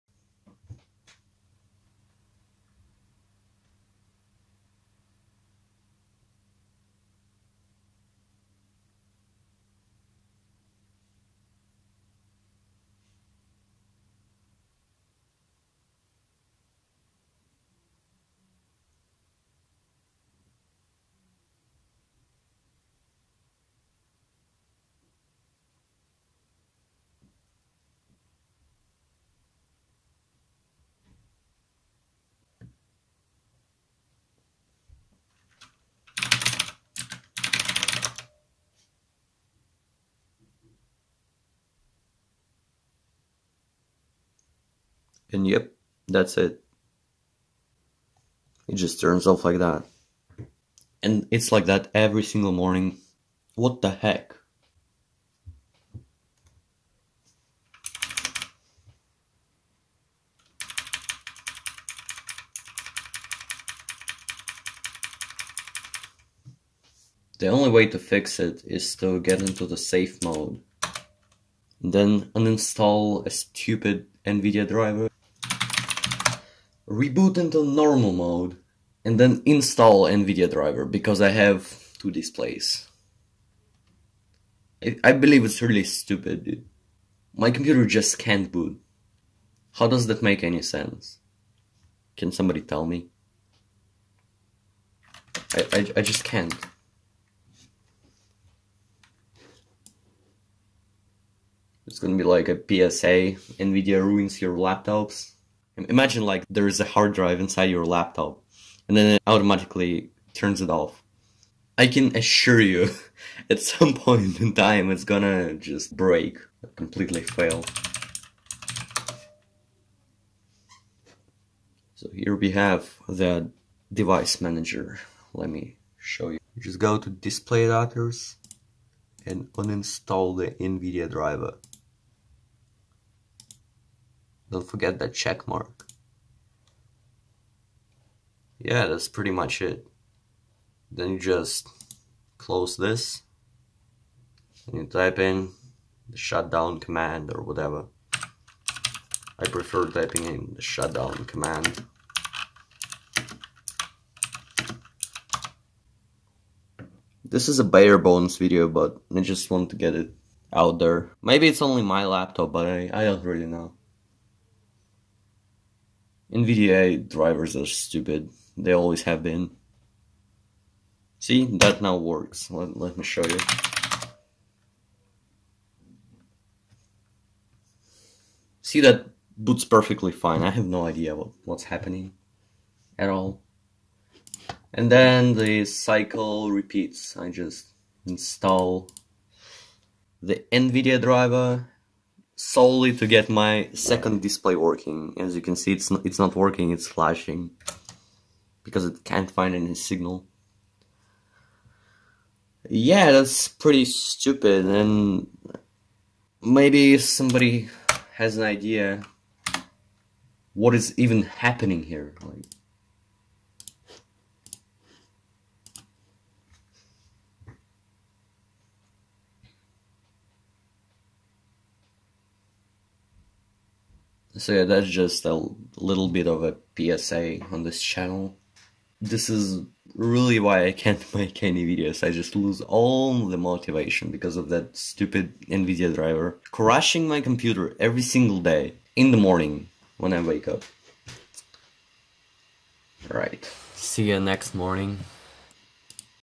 NVIDIA is always the reason
Hello, my friends! Let's hit 10K likes?
When in doubt, always uninstall NVIDIA drivers, every day. Why do you guys think my laptop is doing this every morning but not every reboot? I am honestly clueless. I decided to upload this video on my first channel to put this out for more people to see.

*Timestamps:*
0:00 - Issue
1:00 - Fix
2:37 - Thoughts